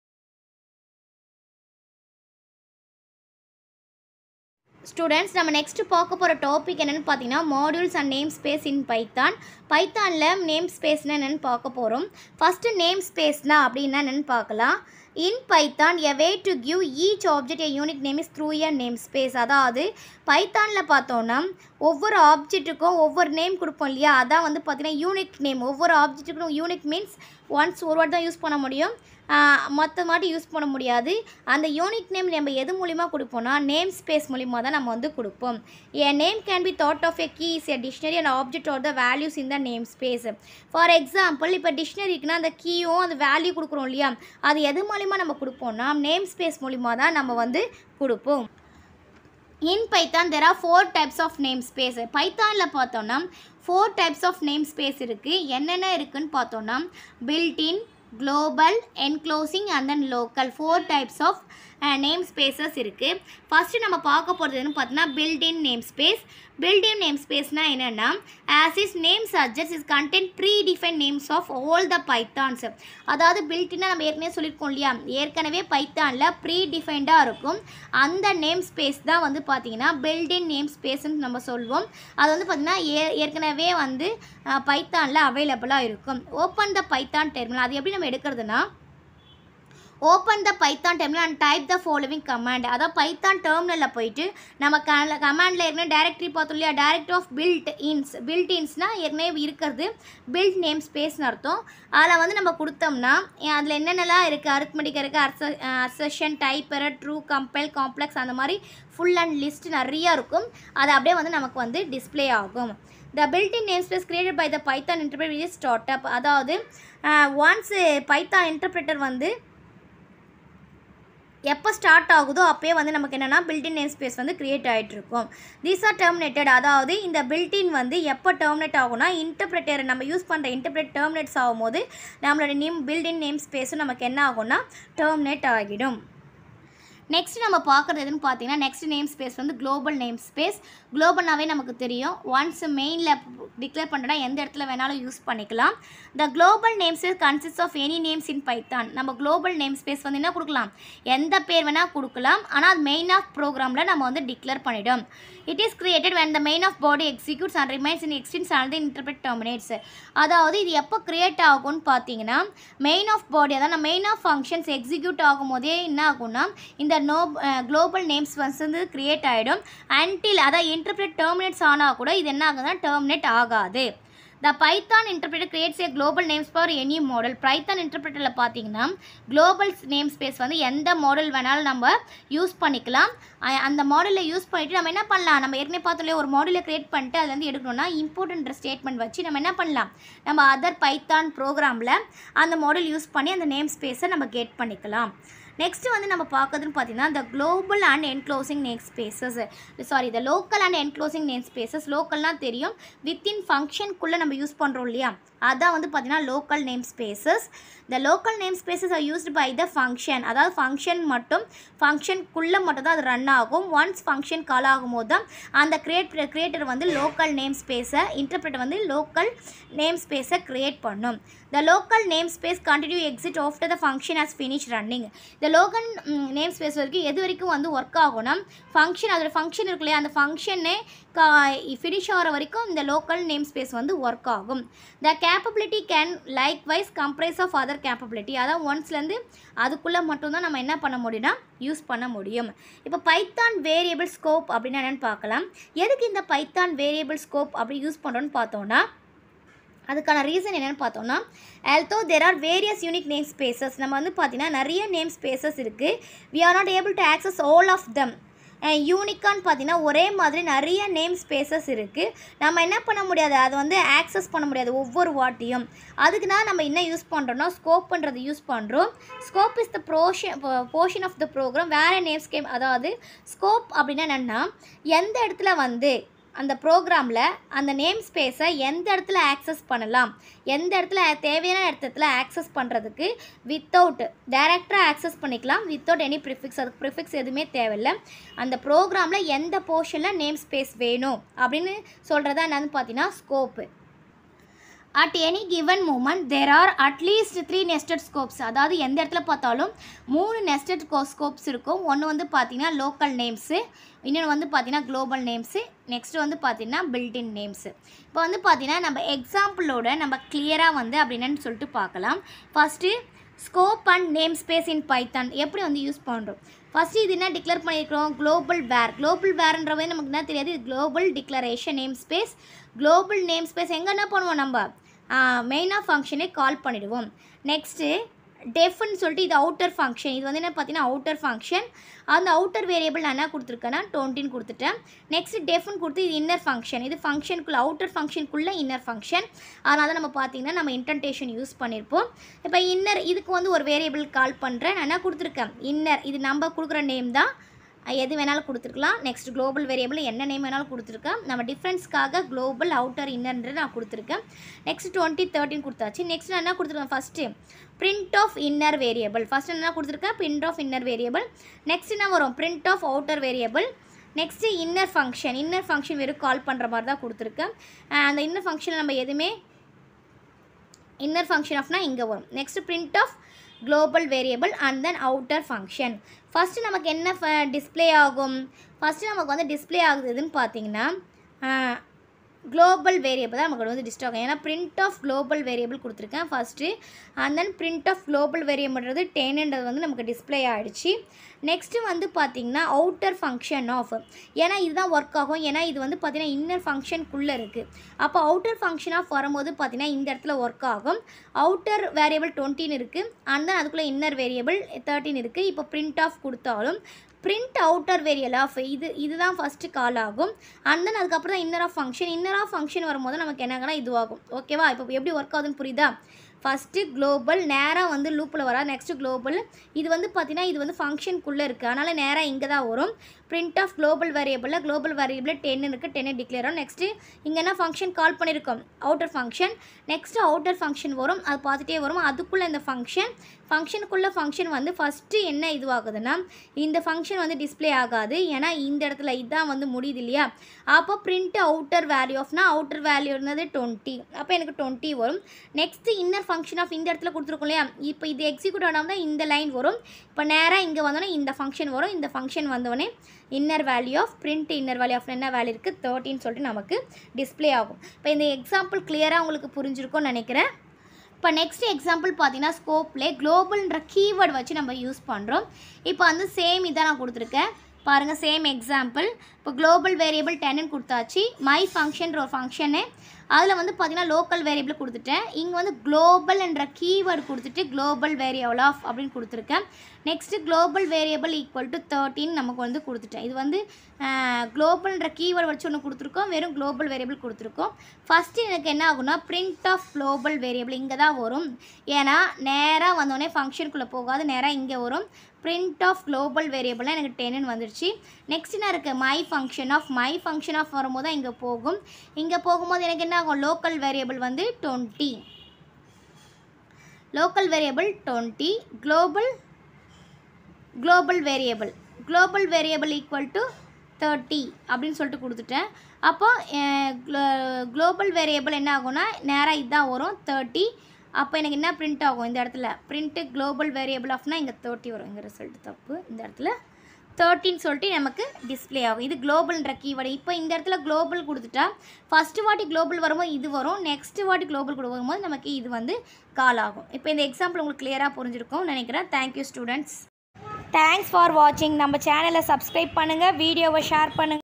modules and namespace in python | python in tamil
This content is about modules and namespace in python with example in tamil